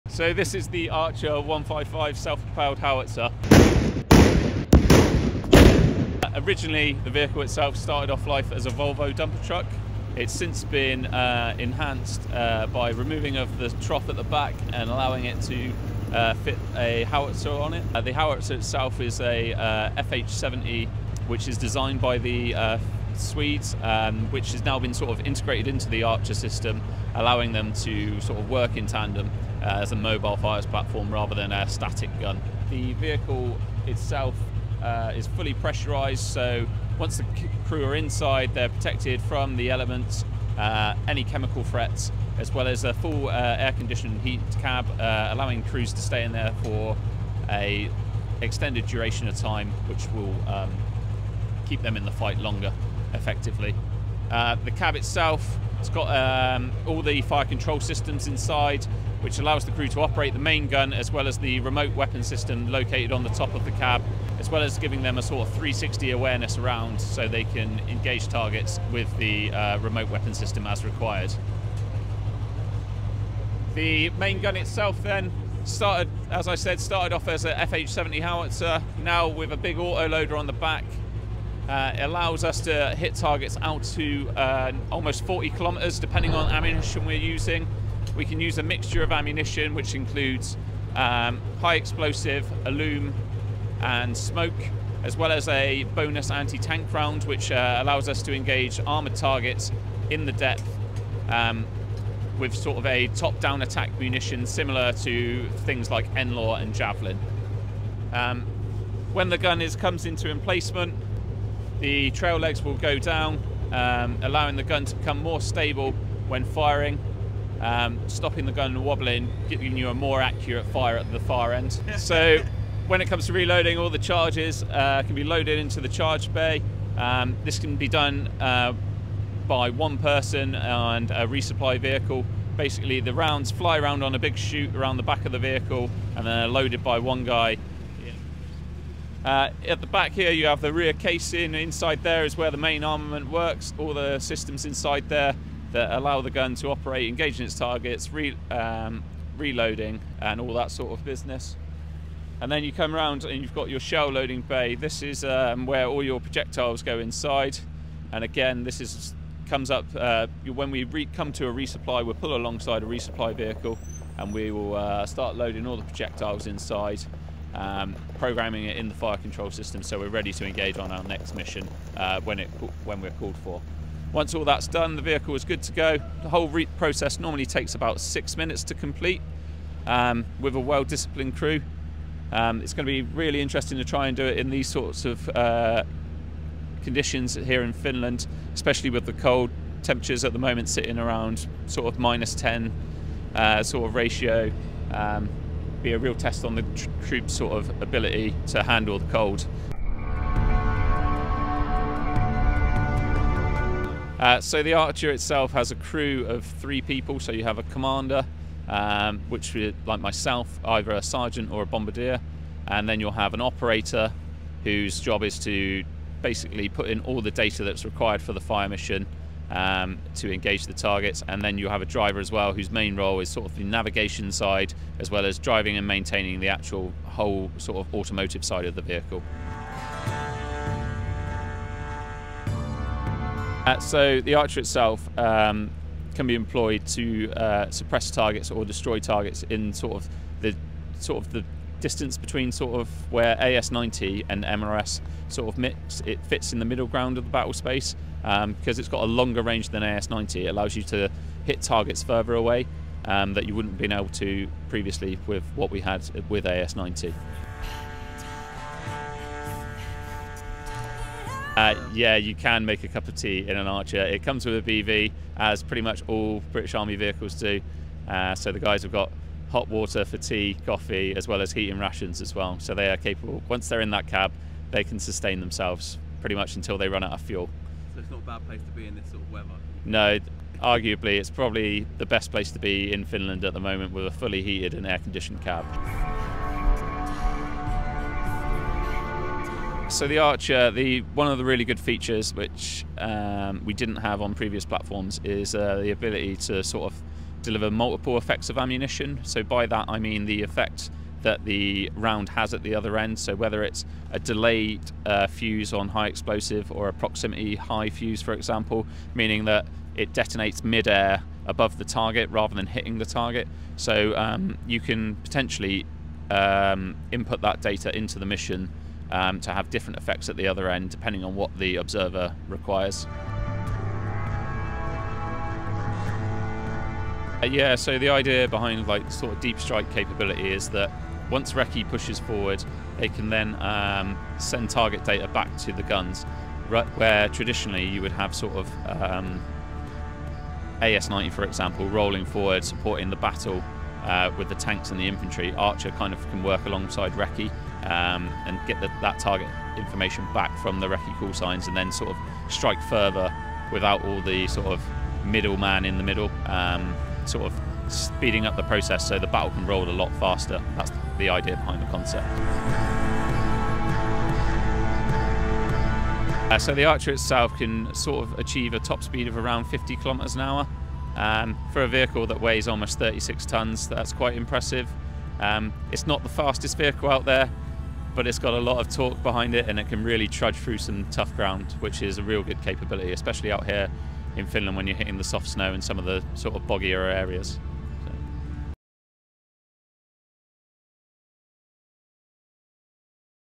British Army Archer Artillery in action: British military testing cutting-edge firepower in Finland
Join Sgt Guest, the Archer Detachment Commander, as he guides us through the impressive Archer Self-Propelled Artillery during Exercise Dynamic Front in Finland! 🇫🇮

With over 250 British soldiers collaborating with NATO forces from 28 countries, this exercise highlights state-of-the-art technology, including the Archer 155mm gun and the Taipan radar system, all in a live tactical setting.  🌍🇬🇧

Chapters:
Introduction: 0:00-3:19
What is the crew size of the Archer: 3:20-3:49
How effective is it on the battlefield: 3:50-4:17
Can you make a cup of tea in the Archer: 4:18-4:45
What is the standout feature of the Archer: 4:46-5:23
What deep strike capability does the Archer have: 5:24-6:02
How fast is the Archer: 6:03-6:28